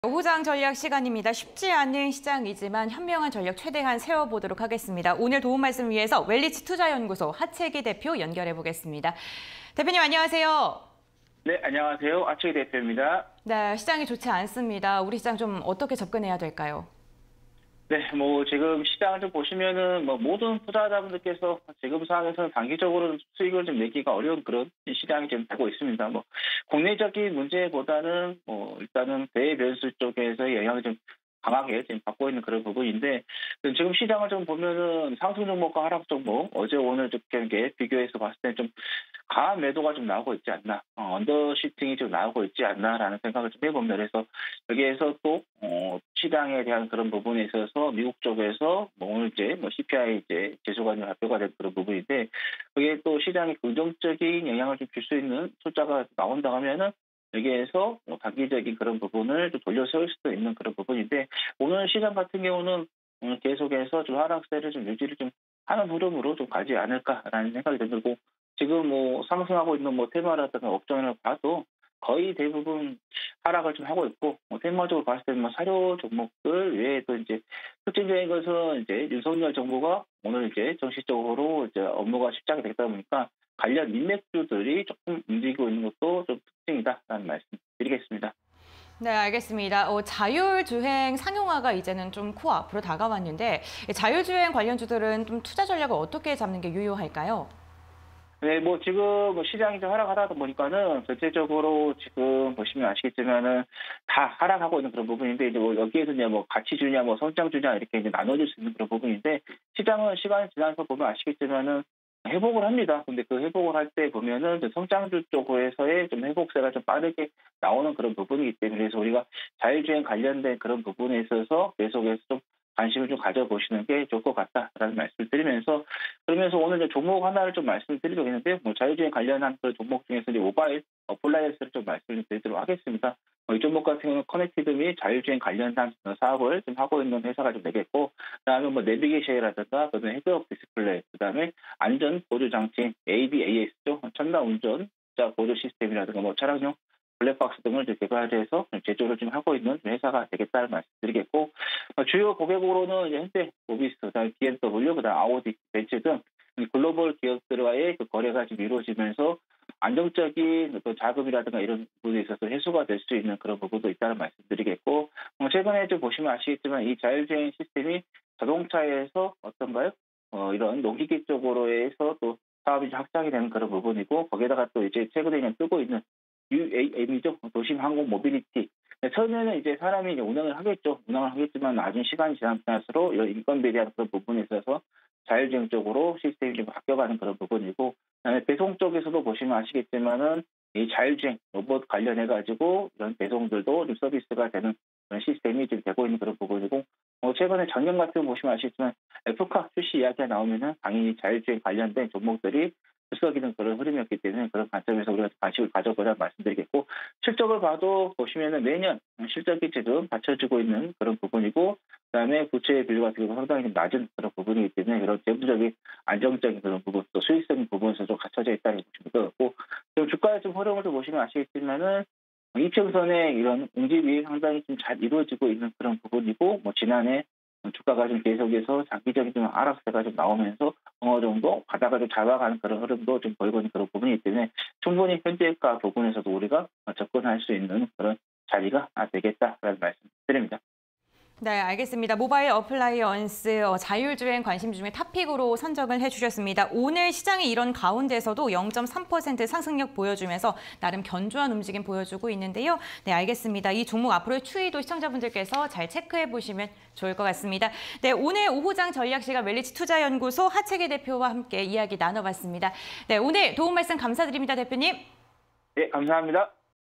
모바일어플라이언스(087260)... 불붙은 자율주행 시장_오후장 전략 (20220510)
Q. 자율주행車 상용화 눈앞, 시장 전망&투자 전략은?
- 자율주행차 시장, 연평균 41%↑ 성장 전망
- 완성차 업체 레벨3 출시 경쟁, 관건은 레벨4 상용화
- 尹정부 미래 먹거리 ‘모빌리티’ 선정
- 2027년 완전 자율주행 상용화 목표
- 상용화에 필요한 인프라·법‧제도·실증기반 마련 약속
- 5년 후 완전 자율주행·3년 후 UAM 상용화 방침
- 자율주행 관련 기술 발전에 탄력 전망
- 해외 레벨3 실제 주행 가능 법률적 요건 구축
- 韓 기술 개발 환경·규제 개선 해외보다 더뎌

모바일어플라이언스(087260)
- 스마트 자율주행 솔루션 전문 기업
- 스마트 자율주행 관련 제품 생산
- 주요 고객사로 글로벌 완성차 업체
- 현대모비스·BMW·아우디·벤츠 등
- 한진-자율주행 로봇 배송 서비스